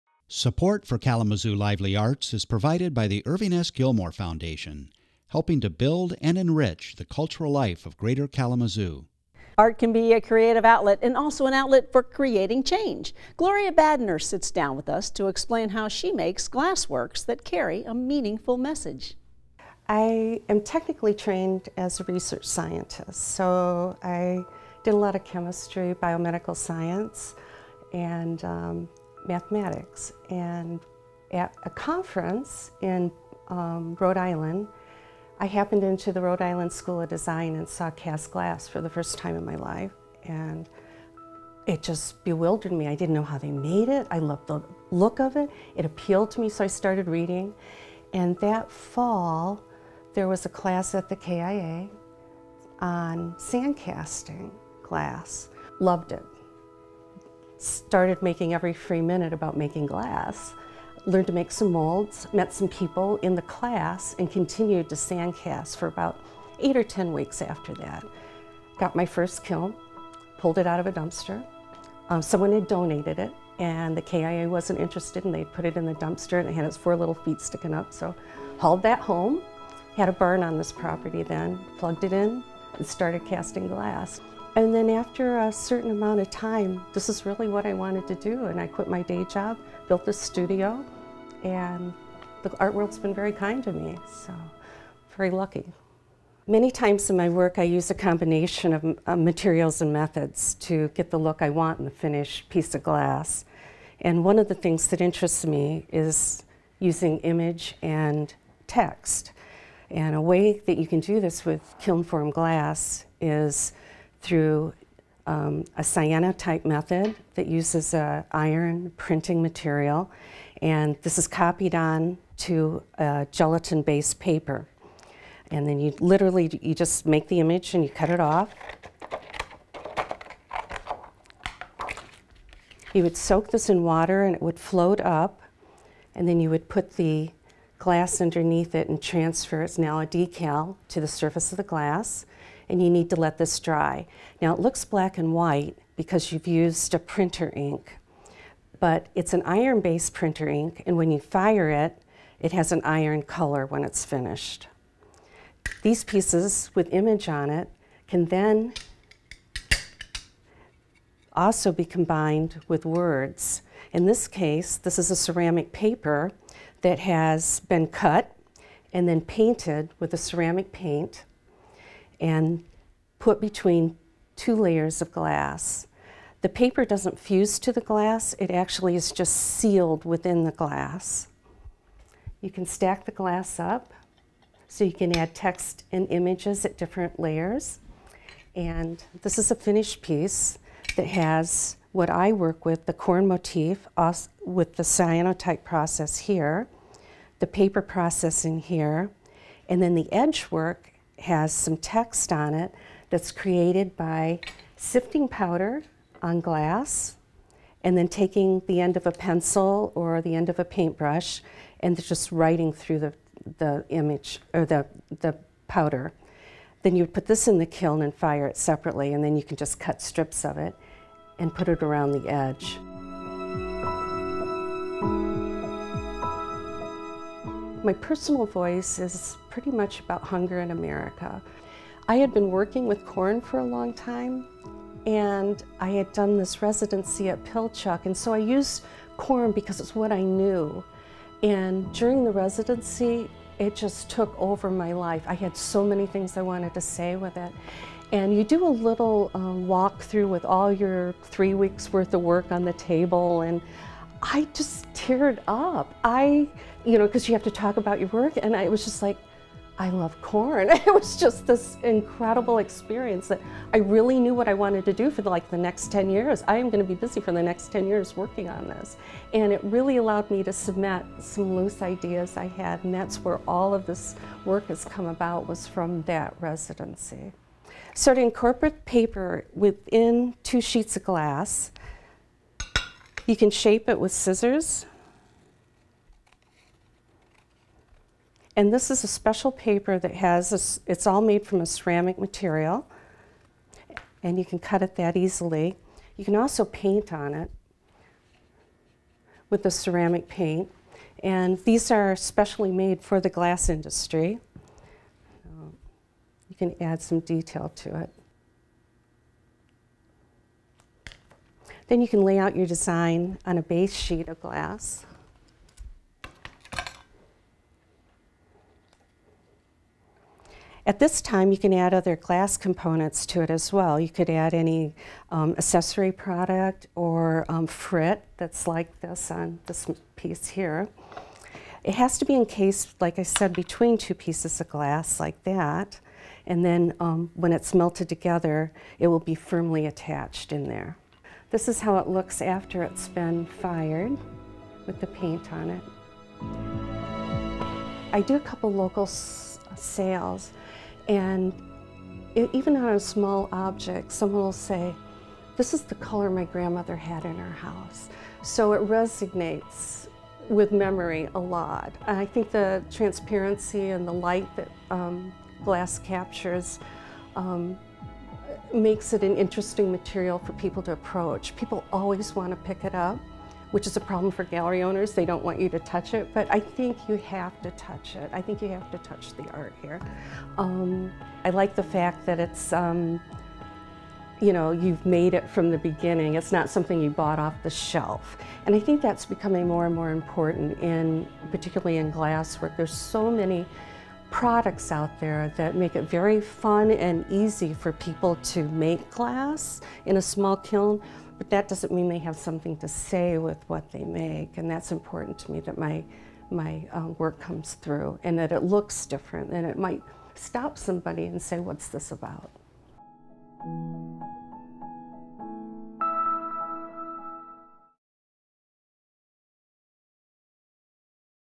Kalamazoo Lively Arts - Arts & Artifacts Glass Studio
Gloria Badiner utilizes an artistic process involving photography and glass!